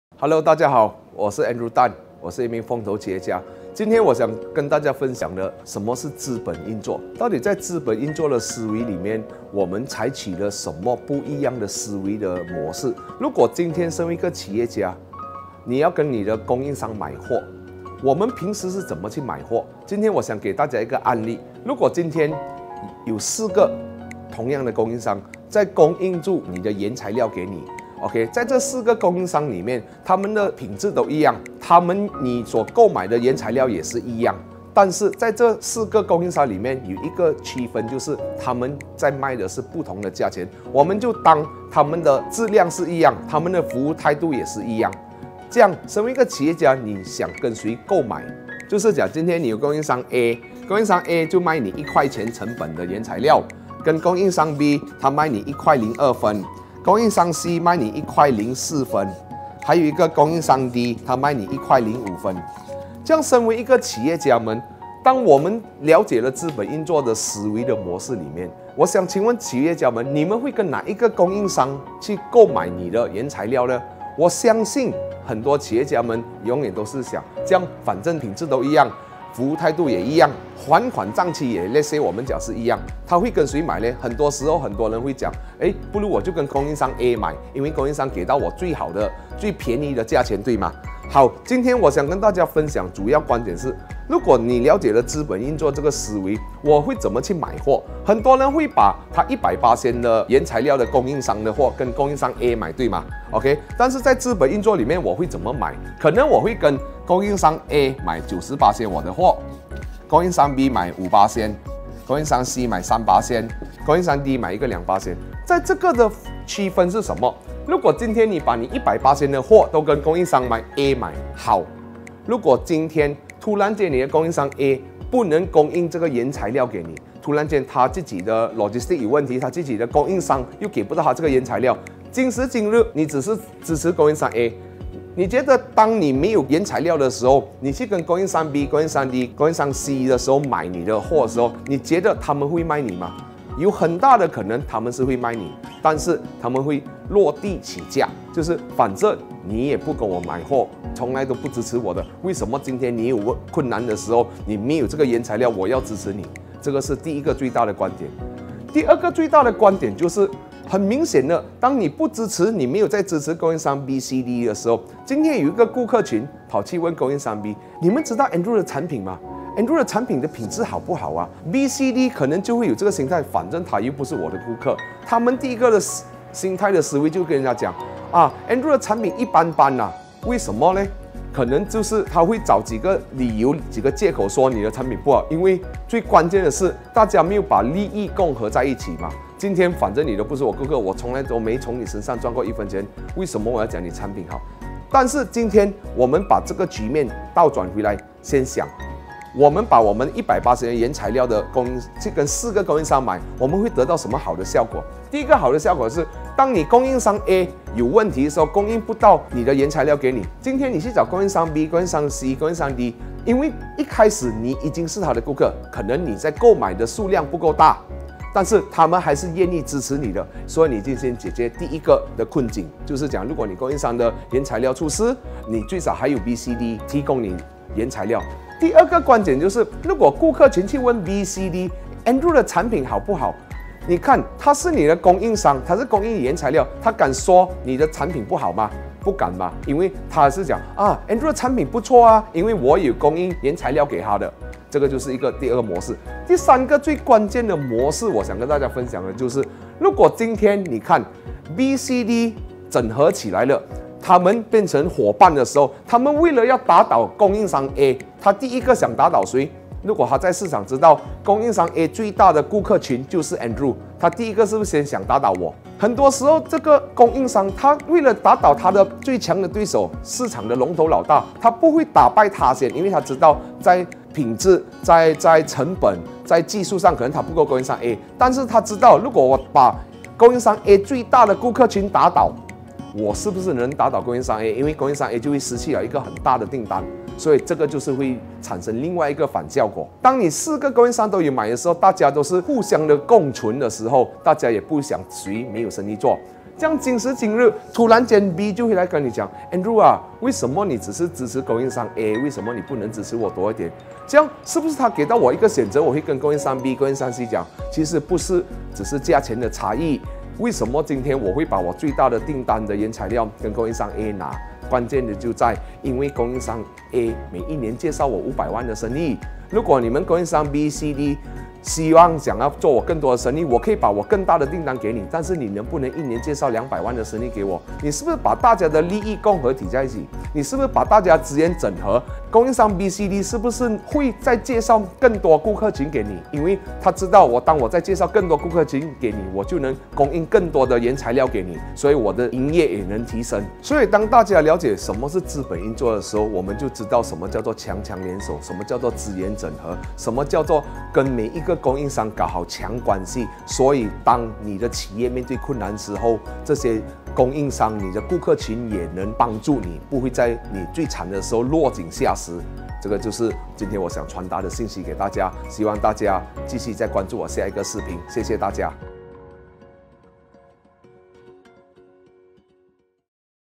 Capital Operation | 资本运作
身为企业家的我们，知道资本运作的思维有多重要吗？

How important is "Capital Operation" to entrepreneurs?